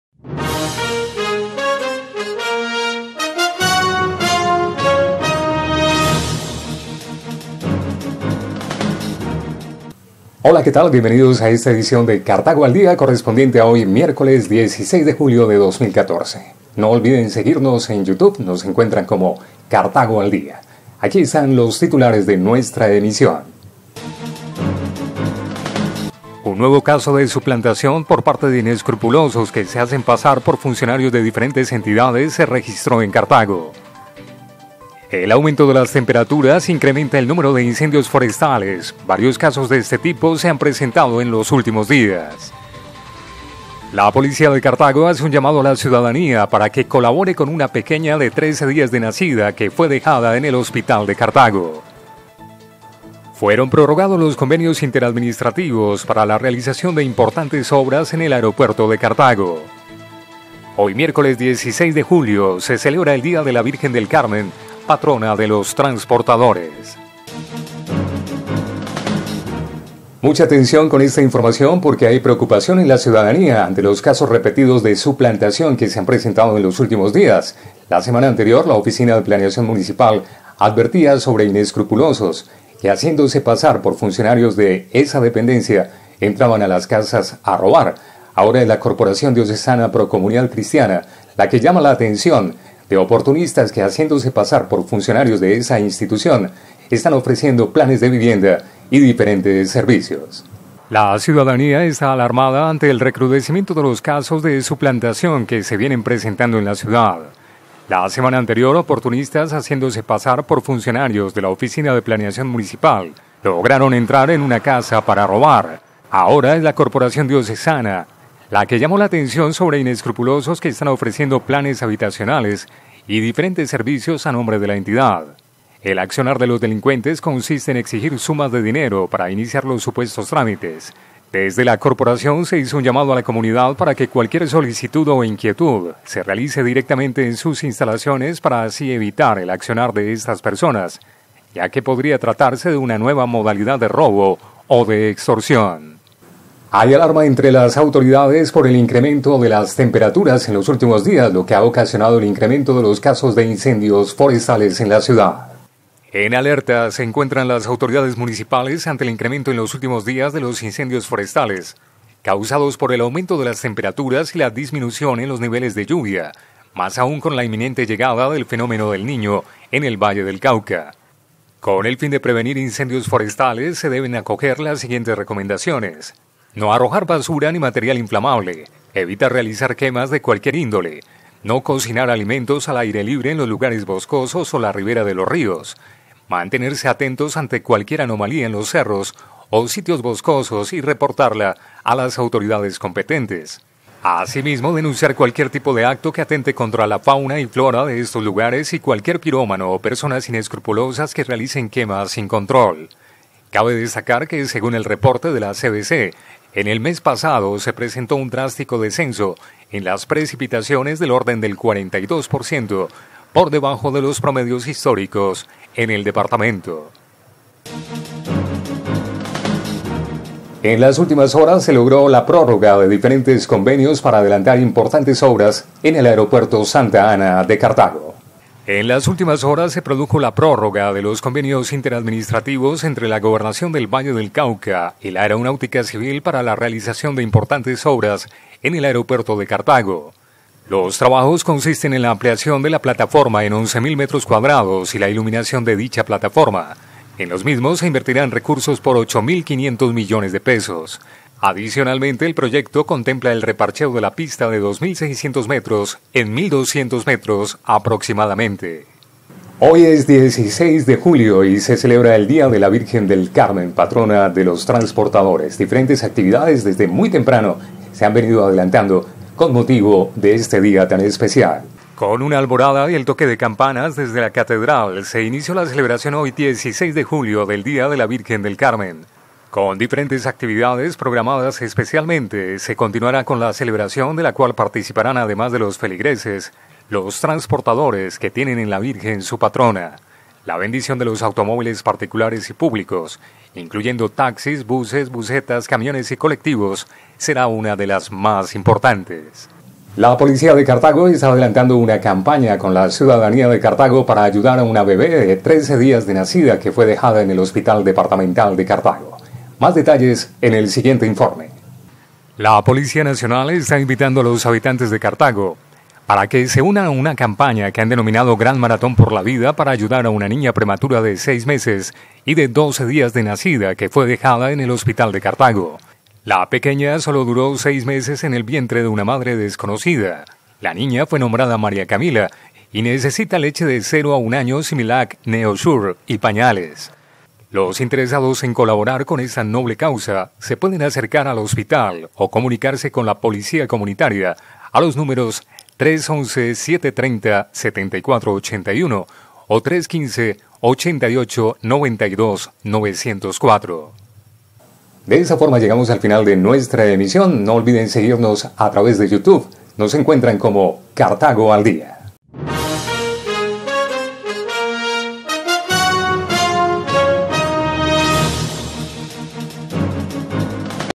cartago al día
En 5 minutos conozca la actualidad de Cartago y el Norte del Valle para hoy Miércoles 16  de  Julio de 2014.